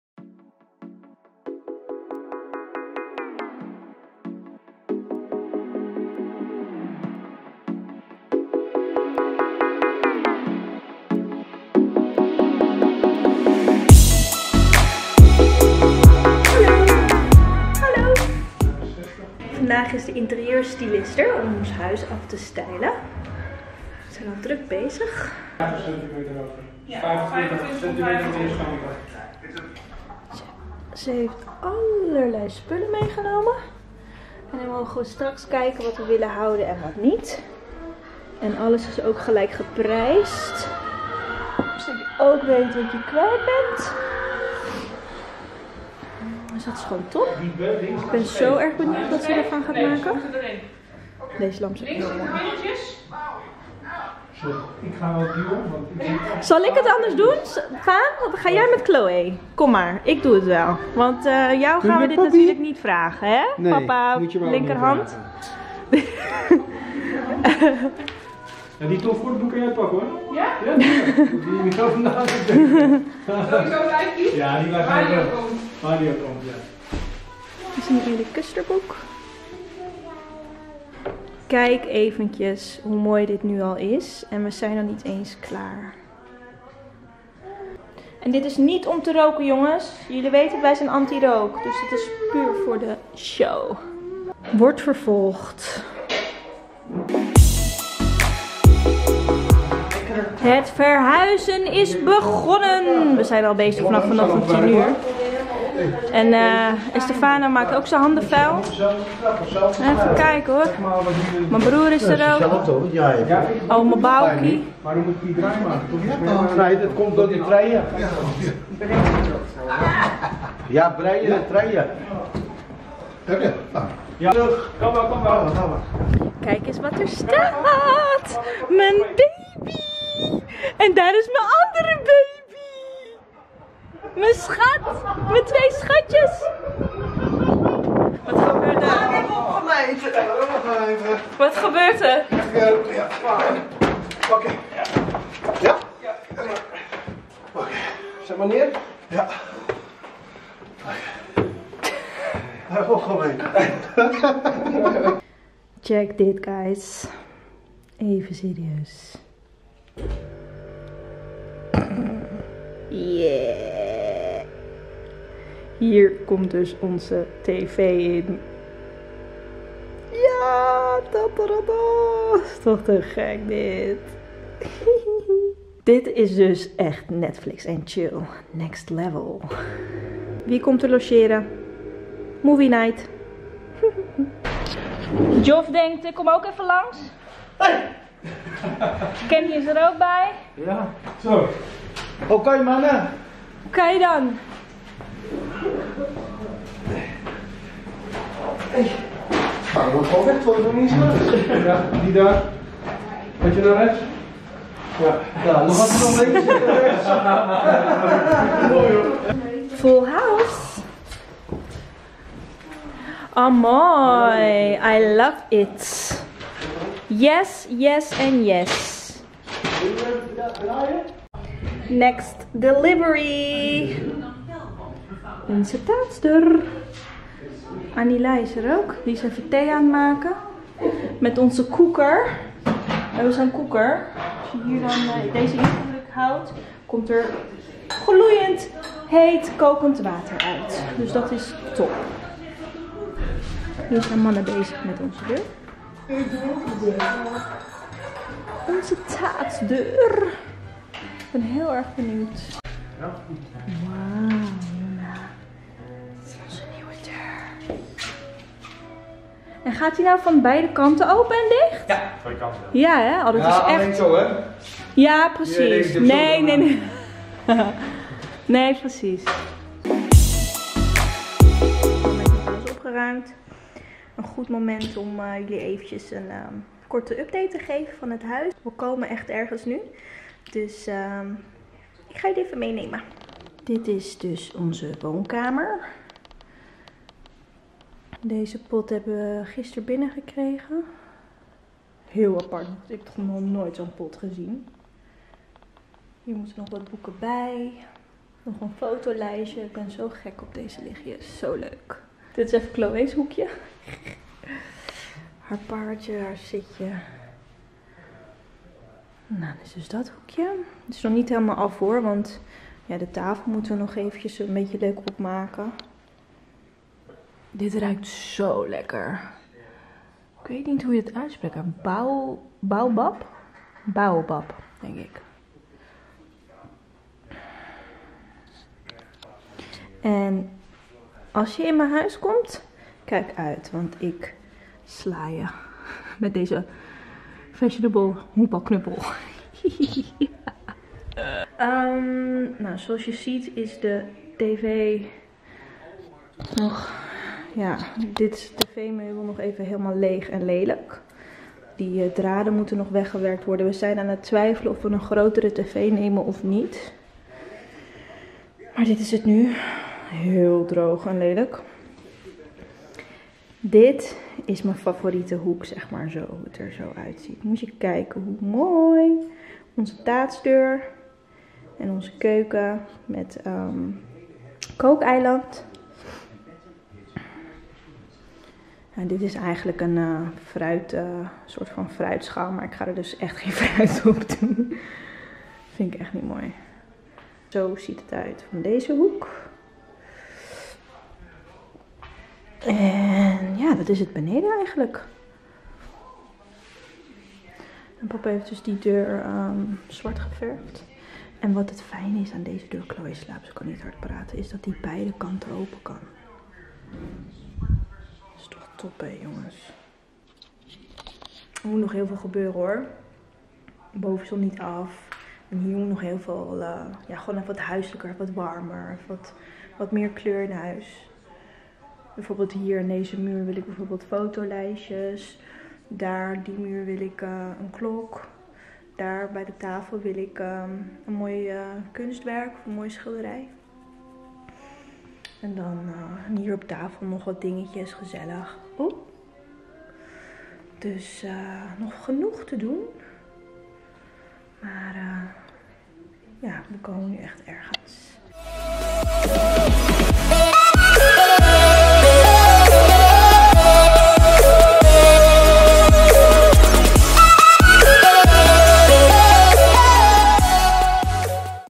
NIEUW BED MET INGEBOUWDE TV.. VLOG #111 -Angela Timisela
In deze vlog gaan we aan de slag met een interieurstylist, wordt onze stalen taatsdeur erin gezet en geniet ik volop van ons nieuwe bed met ingebouwde TV!

Veel kijkplezier,
Liefs Ange X